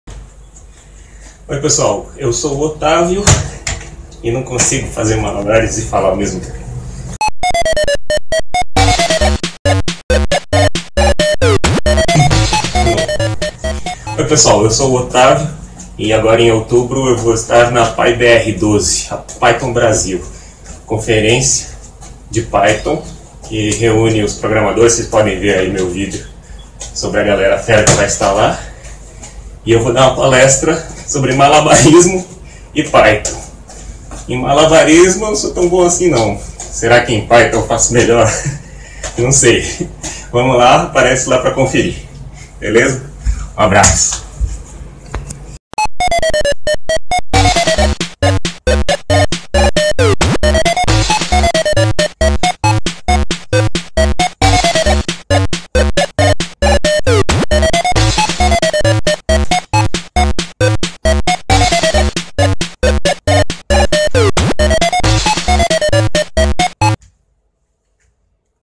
Convite: Python para malabaristas
Otávio convida a todos para sua palestra na Python Brasil 2016, conferência de programadores na linguagem Python. Otávio falará sobre "Python para malabaristas, uma introdução ao OpenCV".